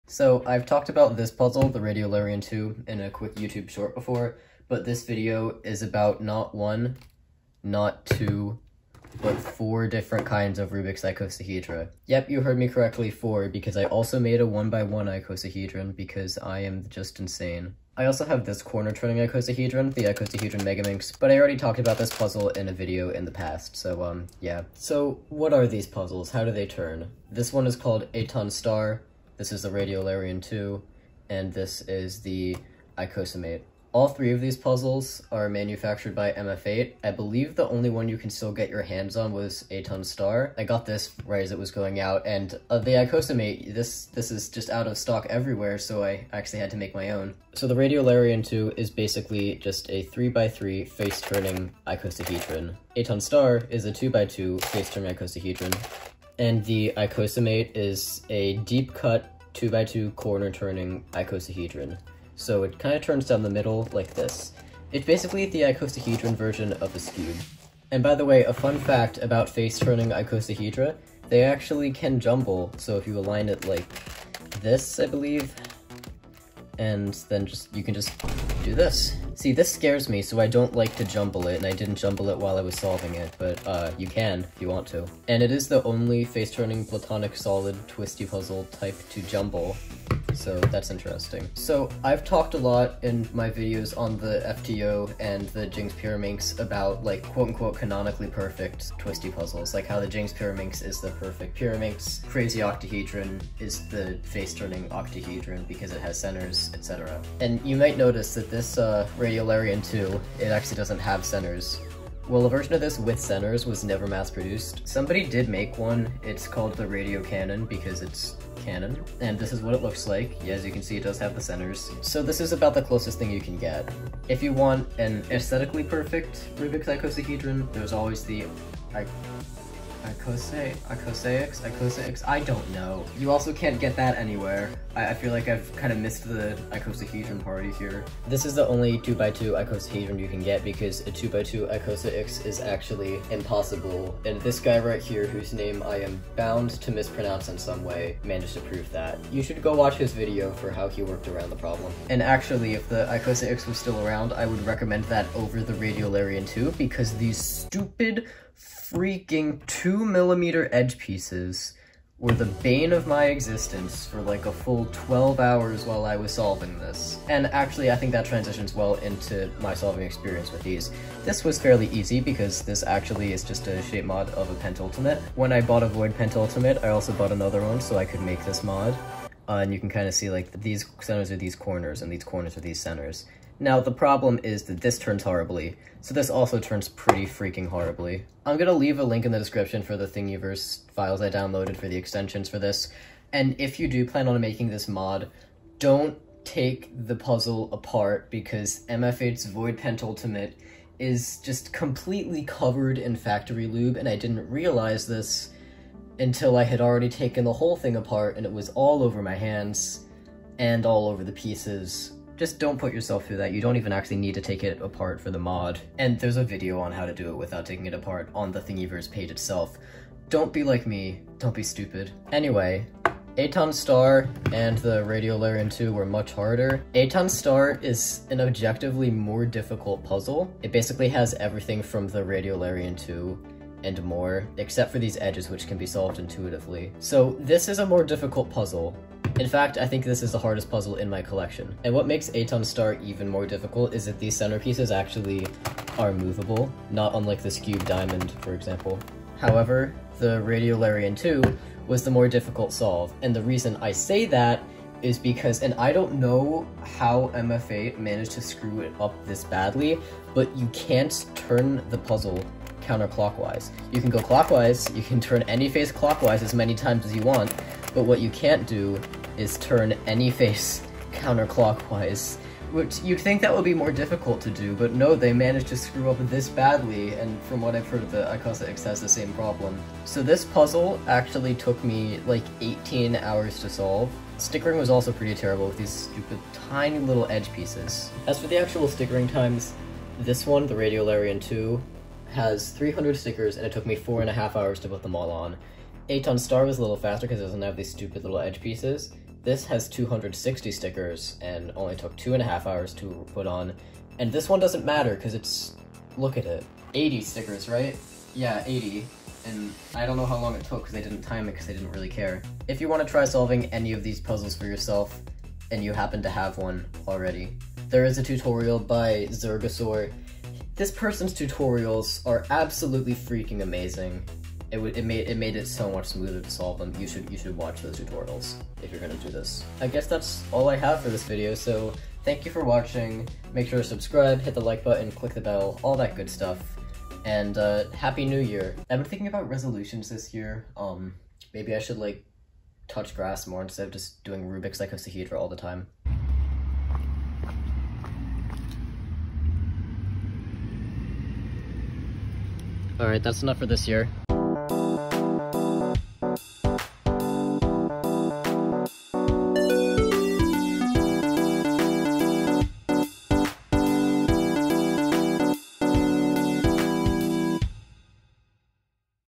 rubik's icosahedra.
In this video, I talk about some icosahedral twisty puzzles, namely the Radiolarian 2, Eitan's Star, and Icosamate.

I am fully compliant with any requests to take down this video.

0:00 Intro
0:24 Puzzle Theory
2:32 Aimless rambling lol
5:30 Outro